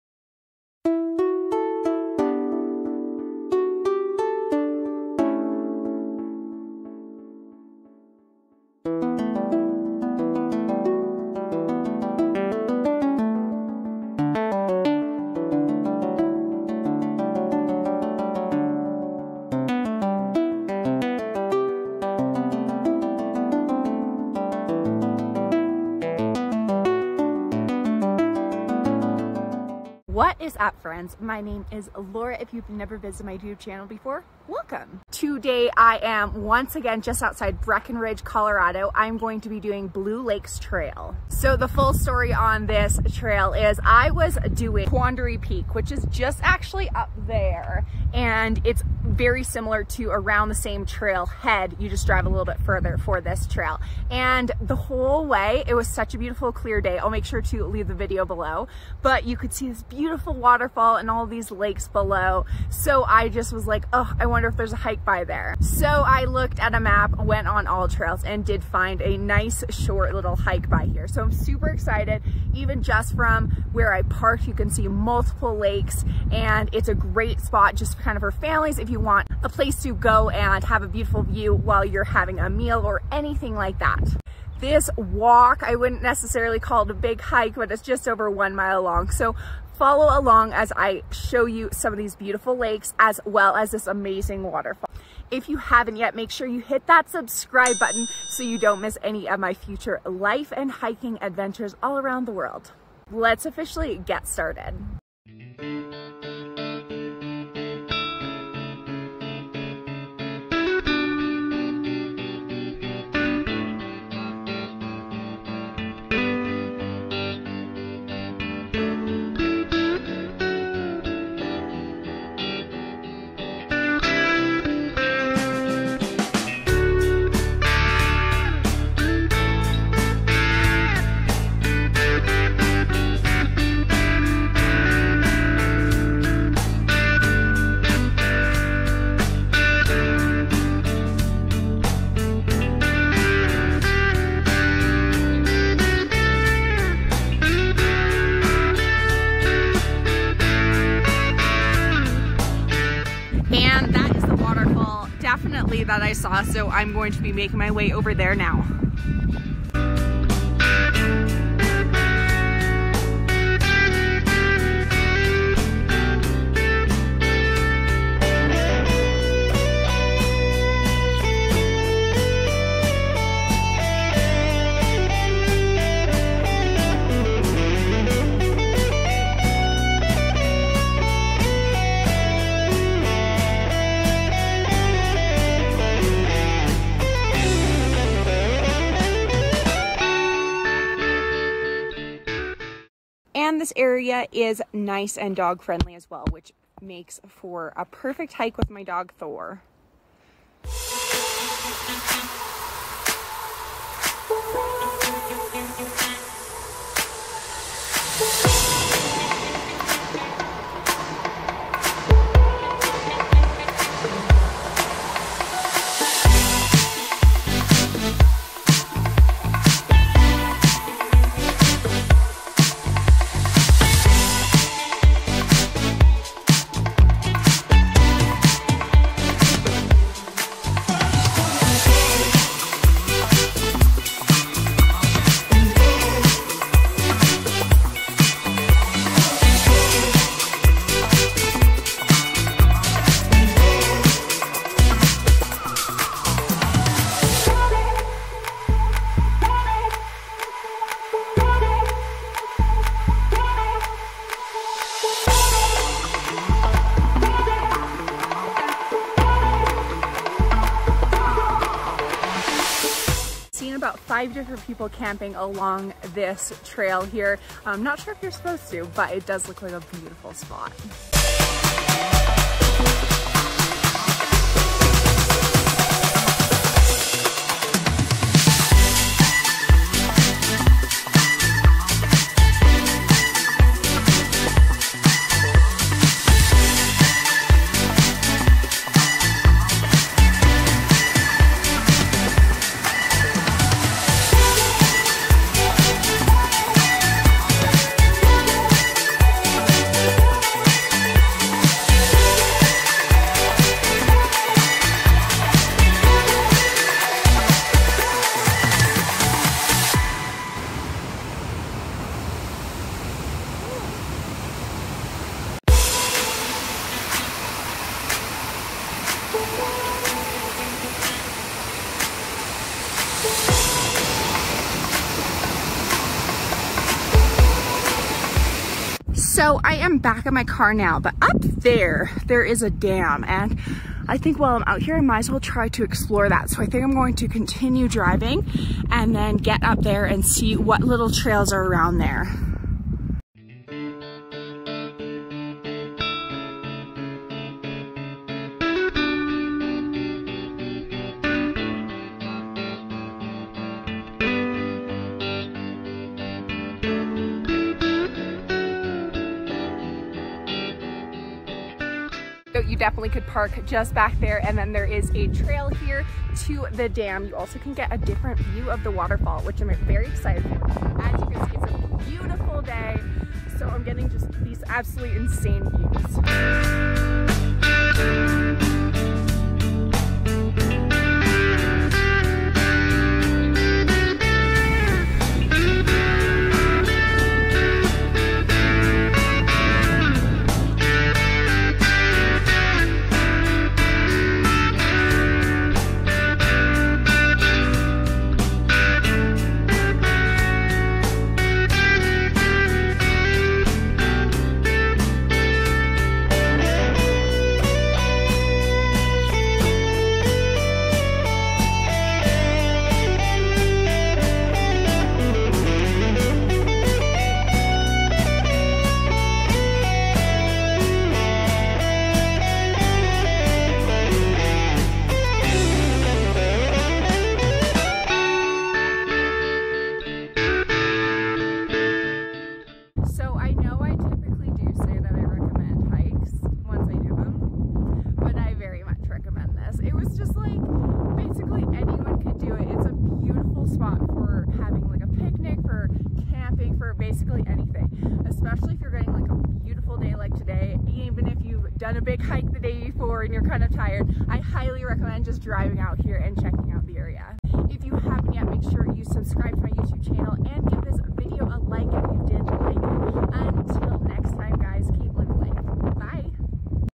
Breckenridge, Colorado | Probably the best short hike I've ever done...
Breckenridge, Colorado offers so many beautiful hikes. Today I hiked Blue Lakes Trail which offers beautiful lakes, a huge waterfall and so much more. This hike in Breckenridge is perfect for taking your breath away and is short so great for all ages!